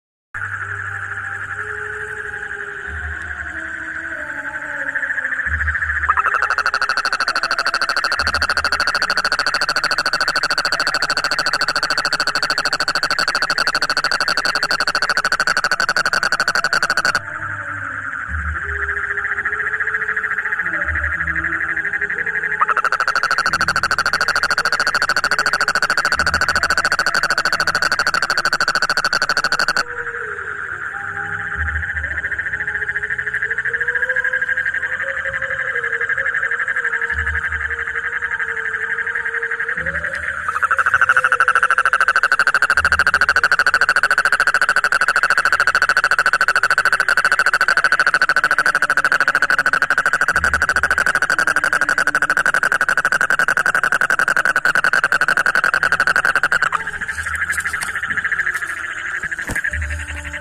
Crazy Indian frog screaming | funny frog sound
Indian tropical frog sqeaking angry. frog screaming outside of a pond in Odisha.